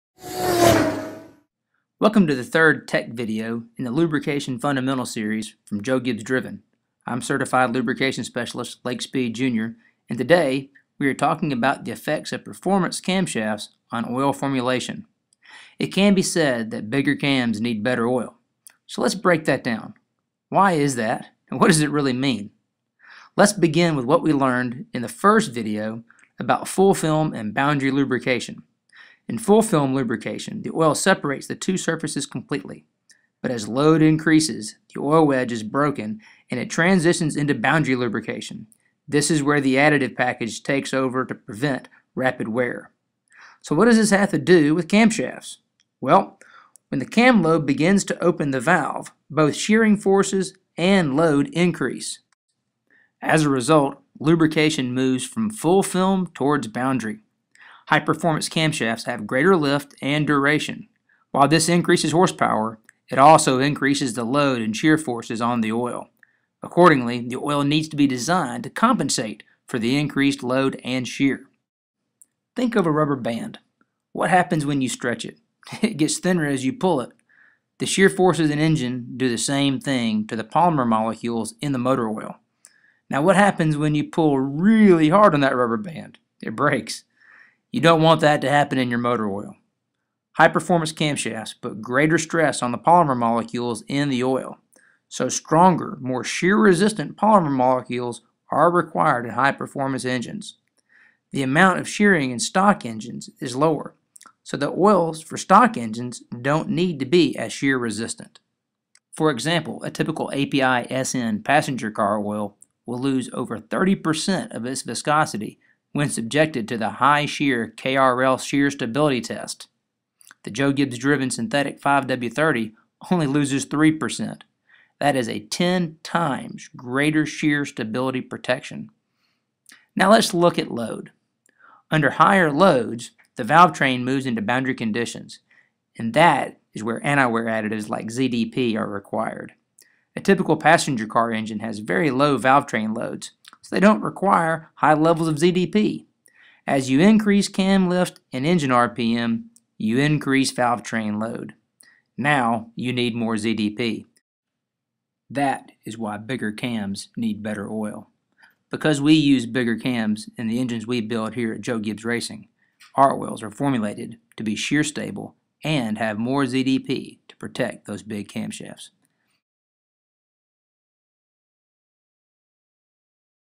Bigger Cams Need Better Oil
Certified Lubrication Specialist, Lake Speed, Jr., explains the effects of high performance camshafts on oil formulations.